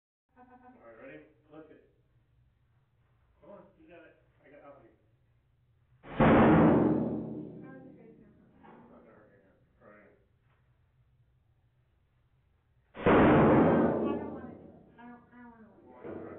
Spouse Smashing @ The Smashit House Davie
Me and my wife breaking shit @ The Smashit House